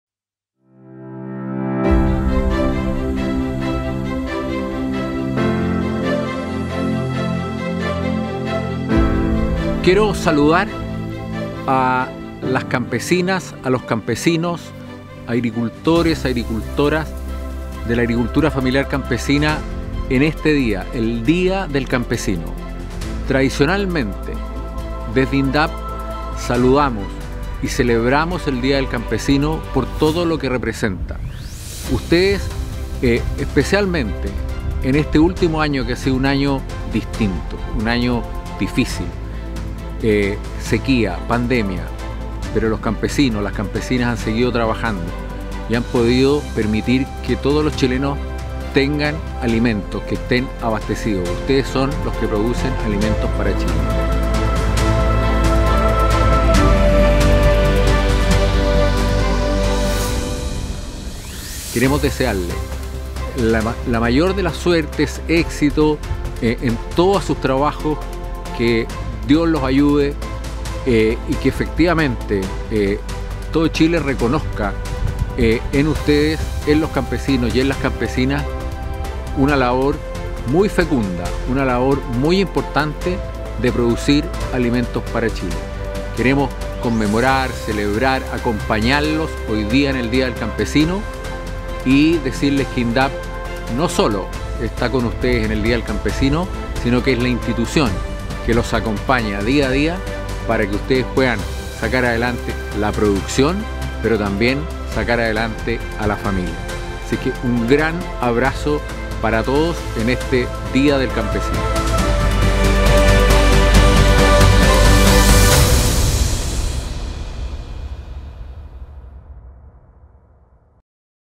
Un saludo especial a los campesinos y las campesinas en su día 💚
"A pesar de este año que ha sido difícil, los campesinos 🧑🏻‍🌾 y las campesinas👩🏻‍🌾  han seguido trabajando permitiendo que todos los chilenos y chilenas tengamos alimentos, que estemos abastecidos. Ustedes son quienes producen los alimentos para Chile 🇨🇱"

Un saludo especial de nuestros director nacional Carlos Recondo para nuestros campesinos y campesinas en su día.
Feliz DíaDeLosCampesinosyCampesinas 👩🏽‍🌾🇨🇱🧑🏻‍🌾